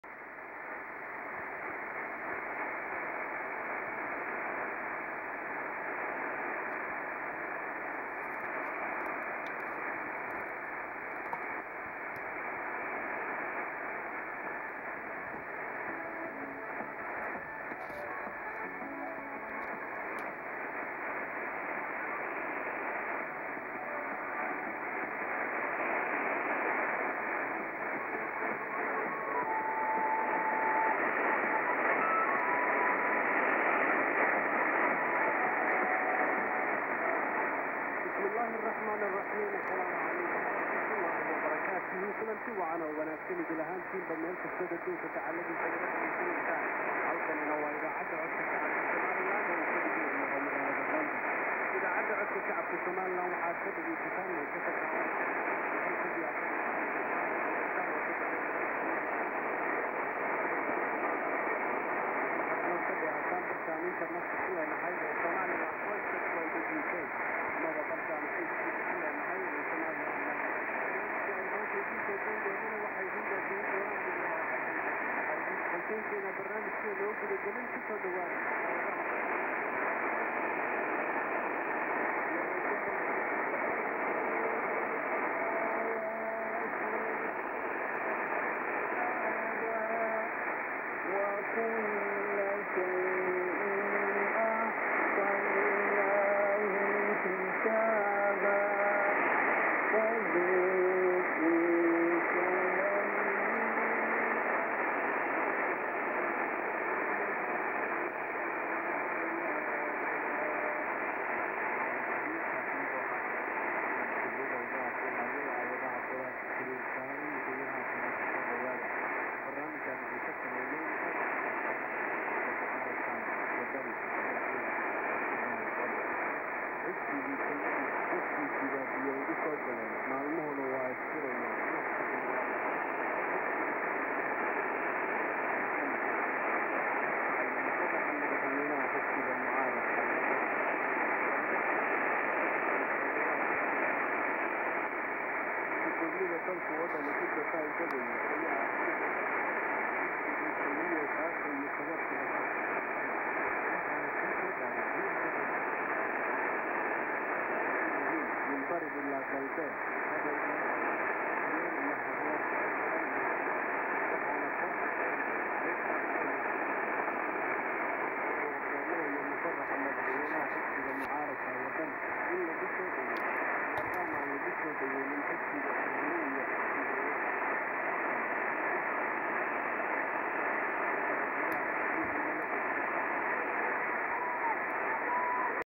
ICOM IC-R8600 9600kHz Voice of the People of Somaliland
Reception at 18:59 UTC on November 11, 2017 in Yokohama, Japan